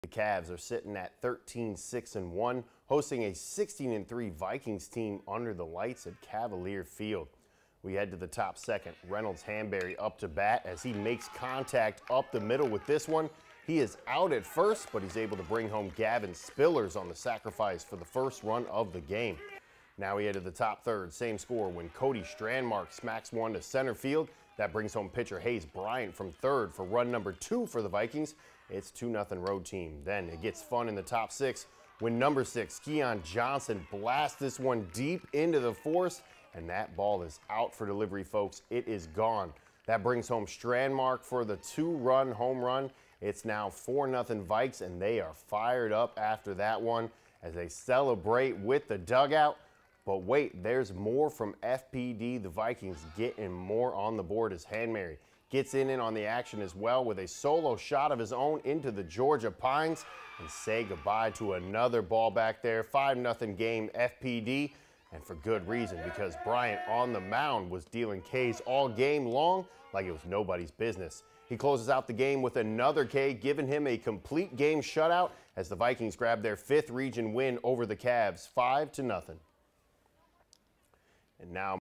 FPD gets the GIAA Region shutout on the road against Mount De Sales
The Vikings pick up their 16th win with a 5-0 shutout victory over the Cavaliers.